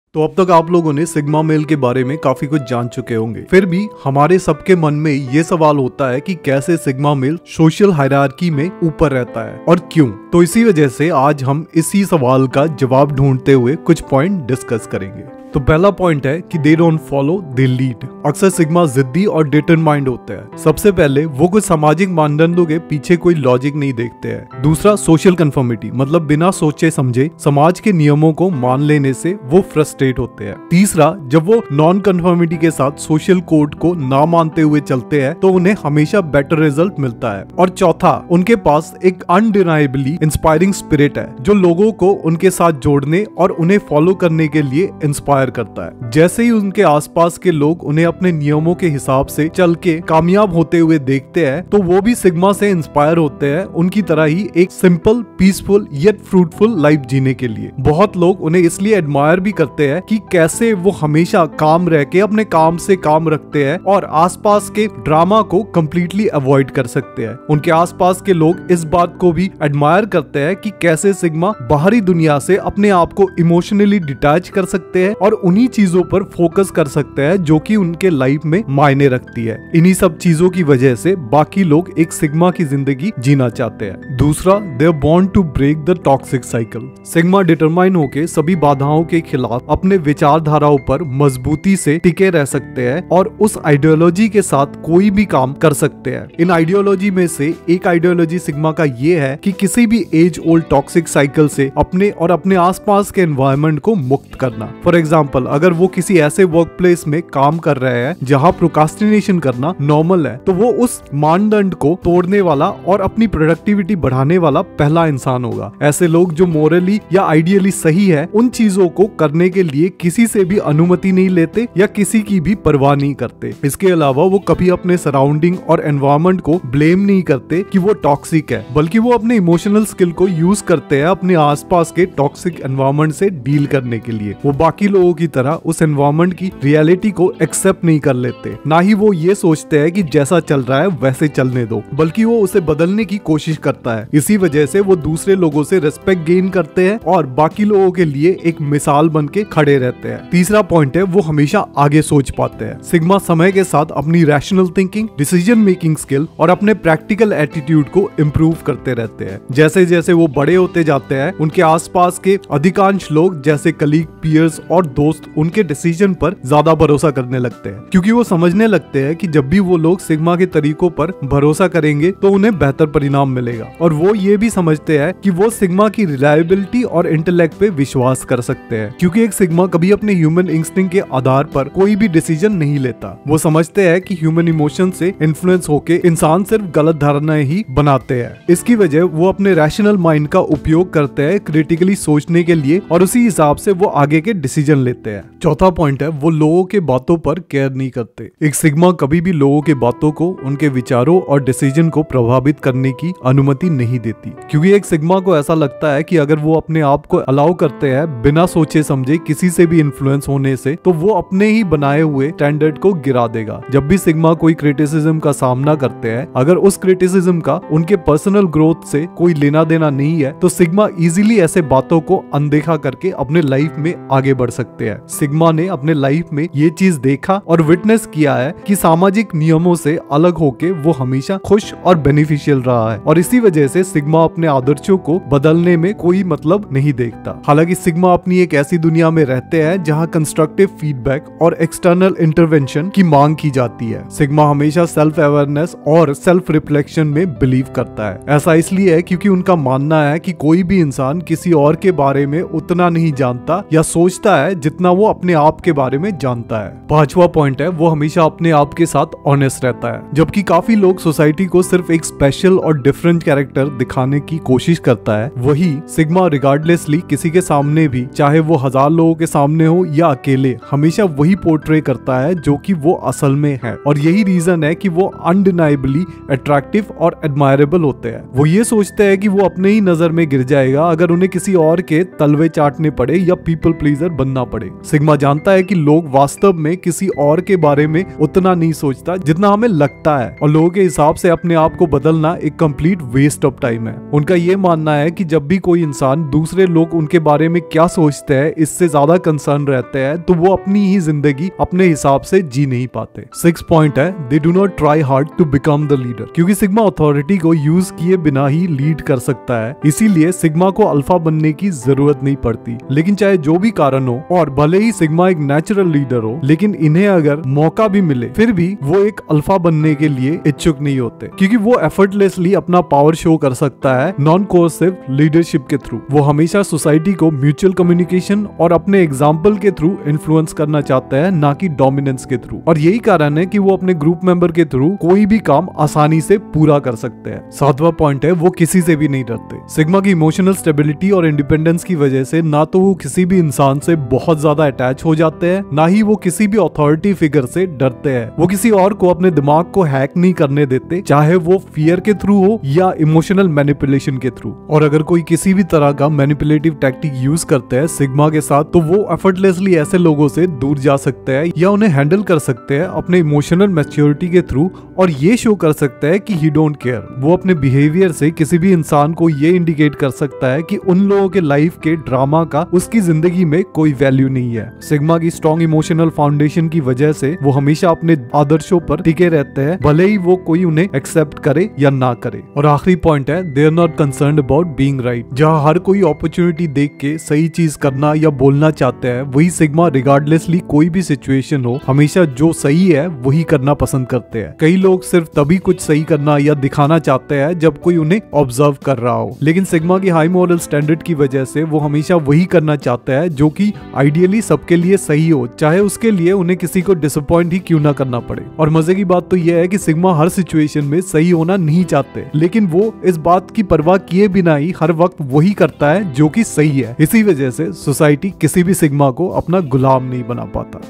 एक सिग्मा Social Hierarchy पर कैसे हावी हो सकता है ? How SIGMA dominate the social hierarchy?
Sigma has a very unique way to climb the ladder of social hierarchy. In this video, we will discuss how a sigma male can dominate the social hierarchy and stay at the top of the social hierarchy.